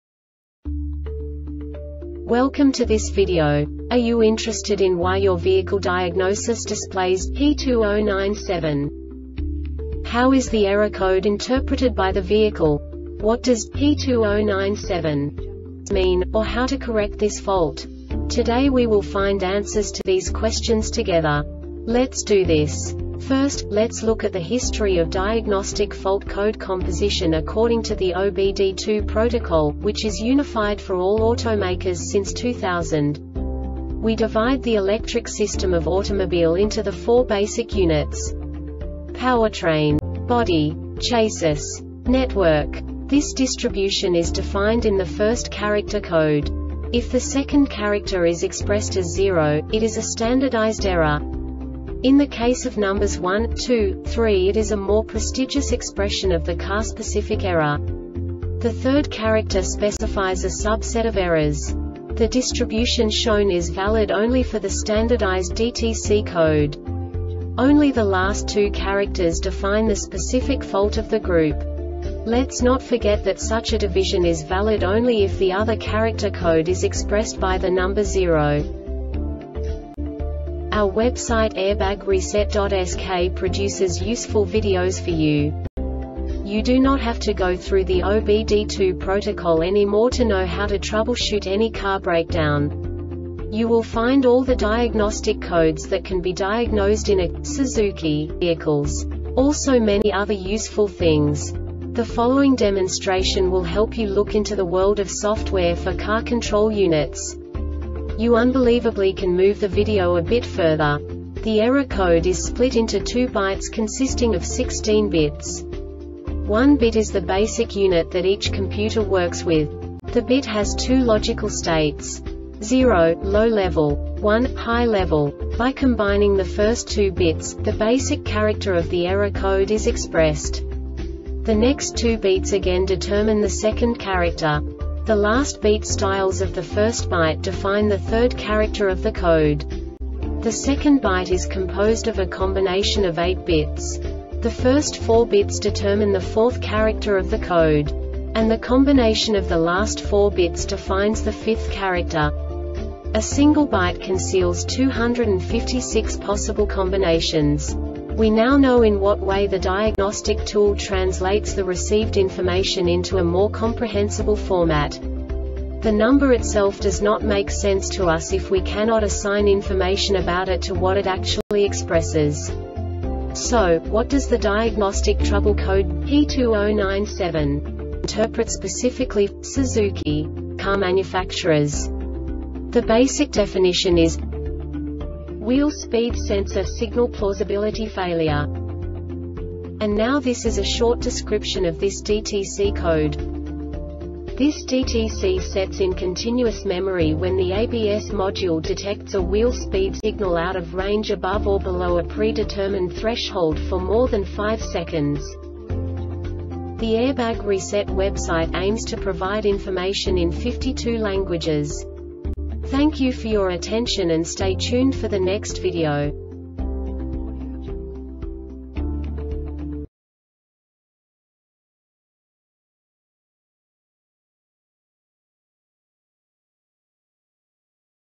DTC Suzuki P2097 Short Explanation
Contents:
0:21 Basic DTC analysis according to OBD2 protocol standard.
1:48 Insight into programming
2:58 A diagnostically understandable description of the Diagnostic Trouble Code
3:05 Basic error definition

Make:
Suzuki
Code:
P2097
Definition:
Wheel Speed Sensor: Signal Plausibility Failure
Cause:
This DTC sets in continuous memory when the ABS module detects a wheel speed signal out of range (above or below a pre-determined threshold) for more than 5 seconds.